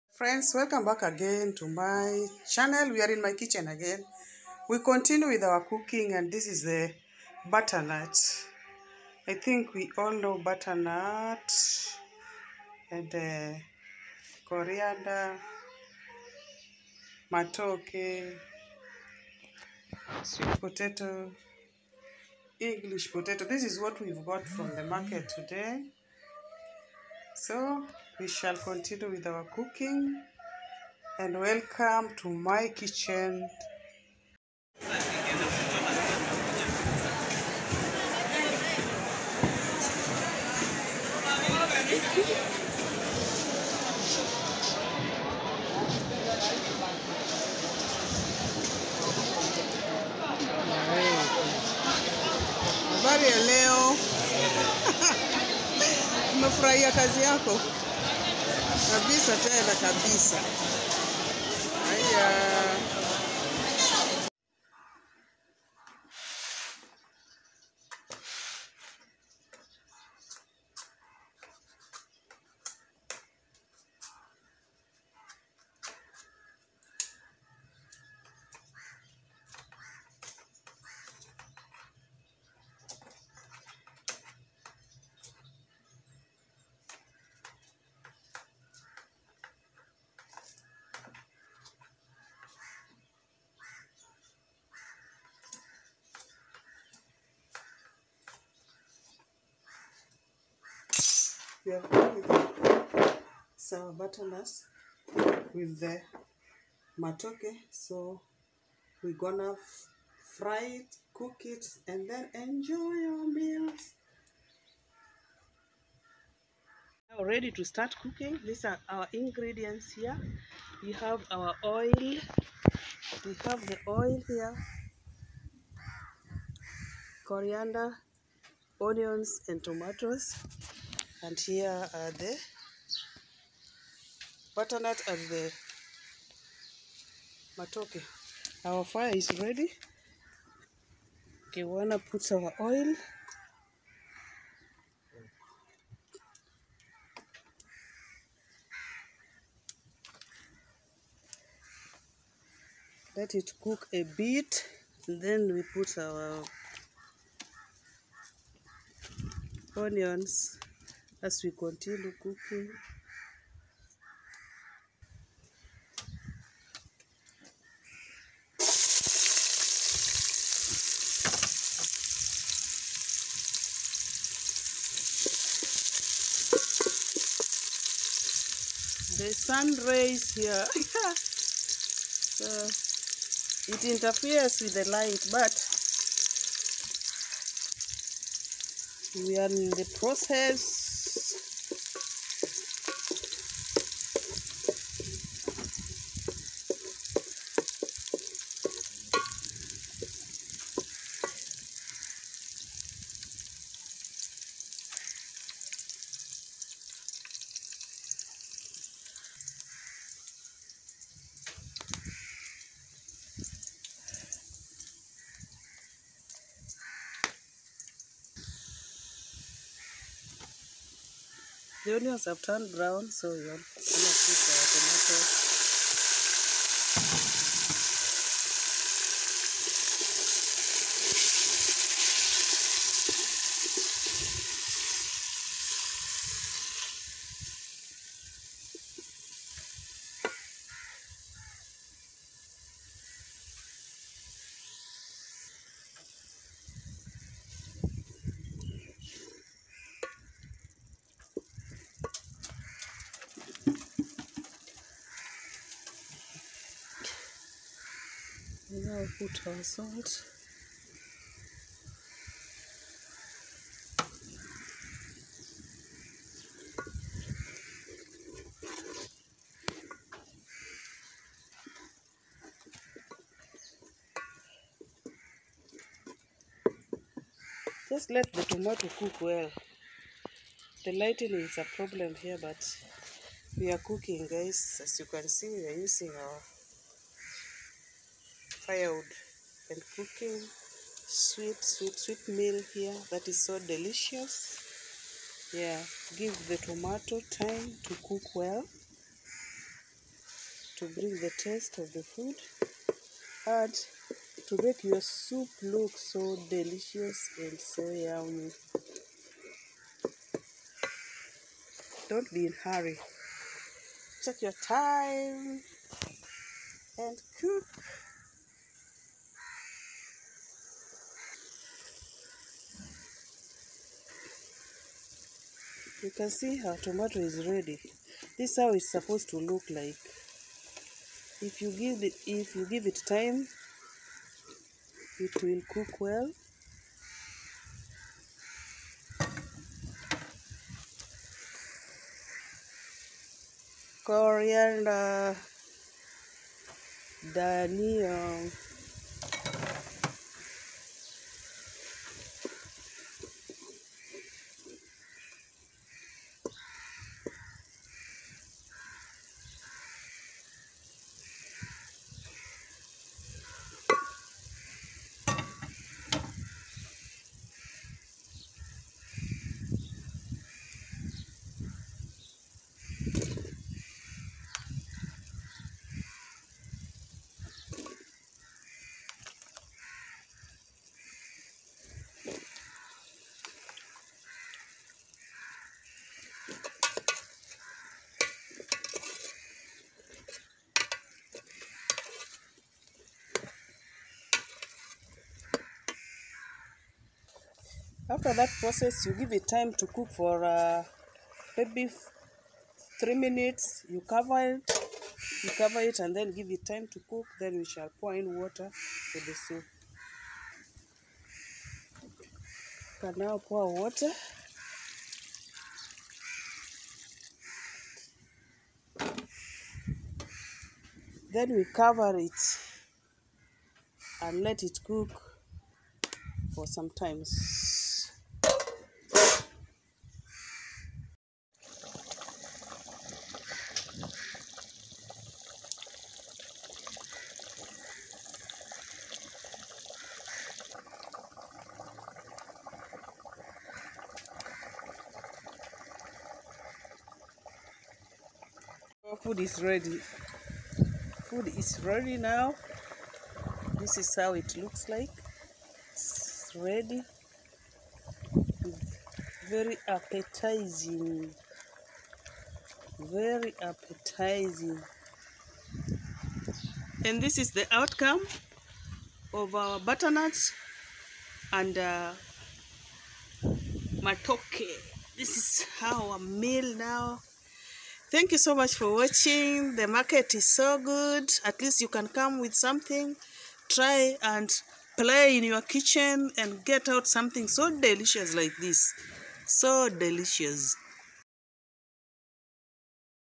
VISITING OUR MARKET AND COOKING THE MOST DELICIOUS AFRICAN FOOD#christiane
VISITING OUR MARKET AND COOKING THE MOST DELICIOUS AFRICAN FOOD.